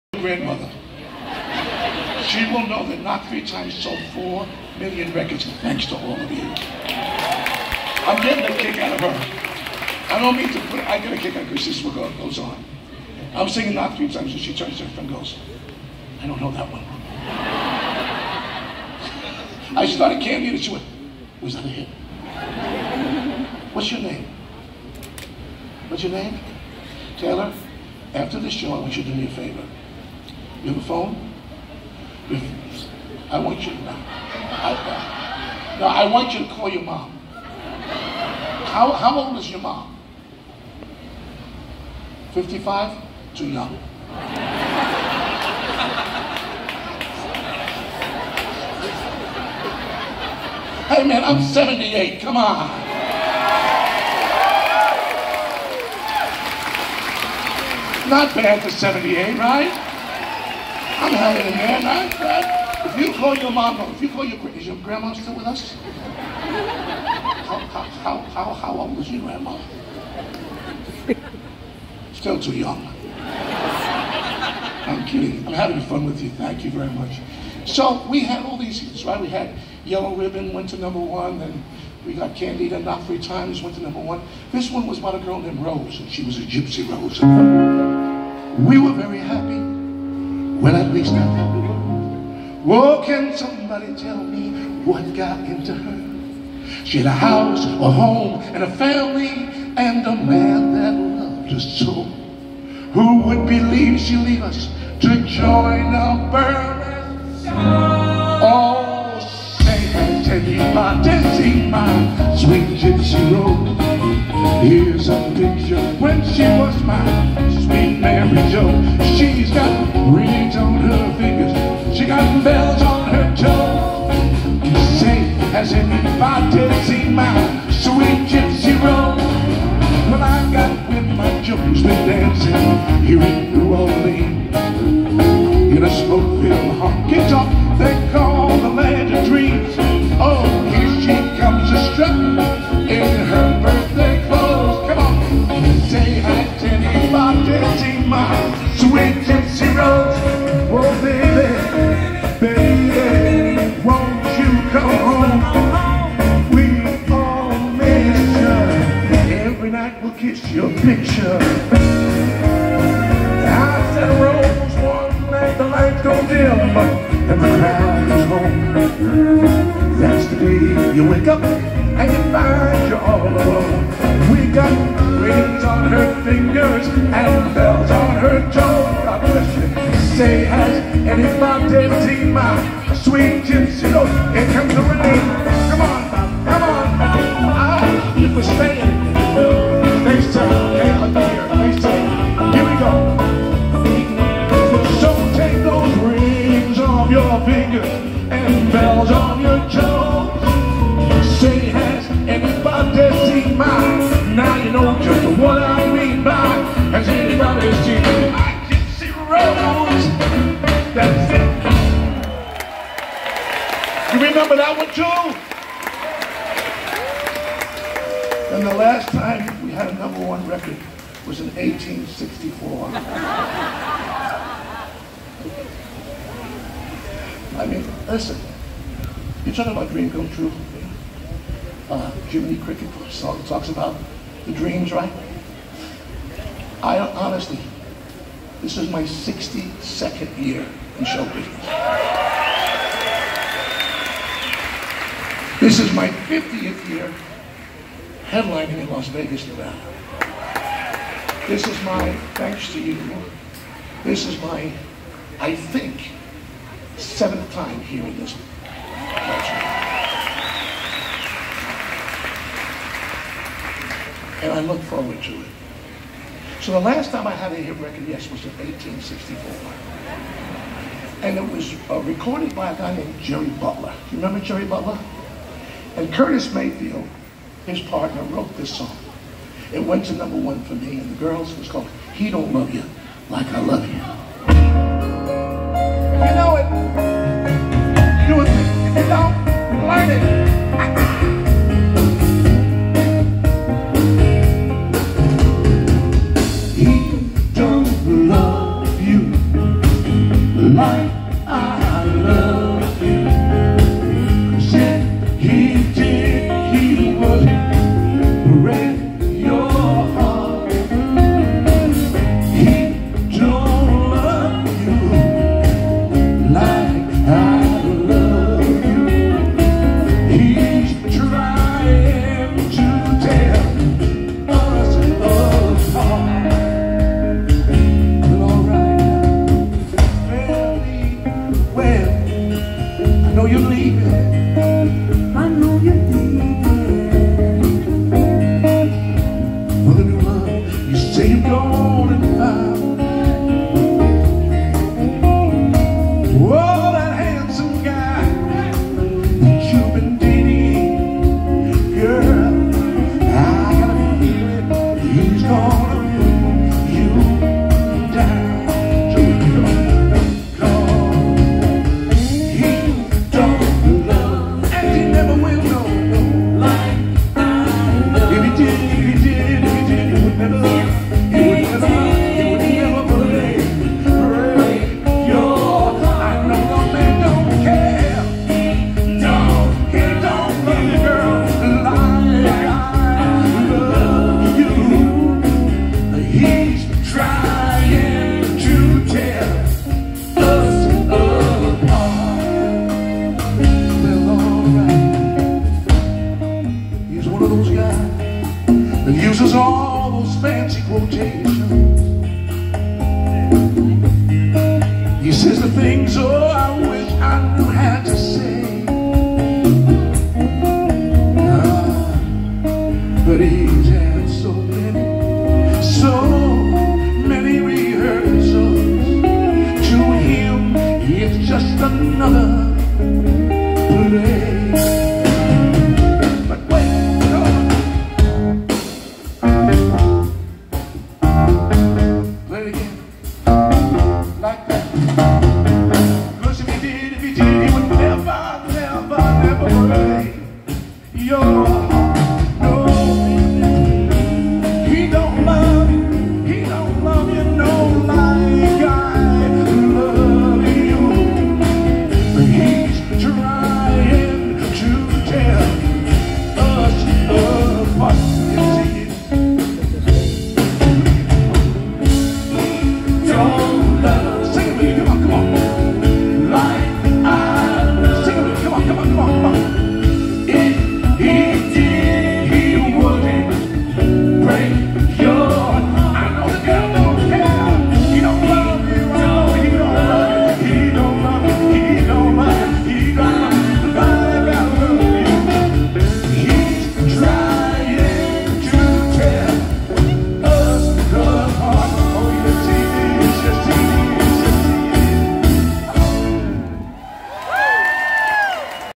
Tony Orlando, Live! Concert at the Garden Rocks Stage in Epcot
Tony Orlando, Live! 5/20/2022 Concert at Garden Rocks inside the American Adventure at Epcot, Walt Disney World Orlando, Florida.

🎤🎸🥁Watch all my live concert footage…

😎Check out some more of my vlogs/travel videos...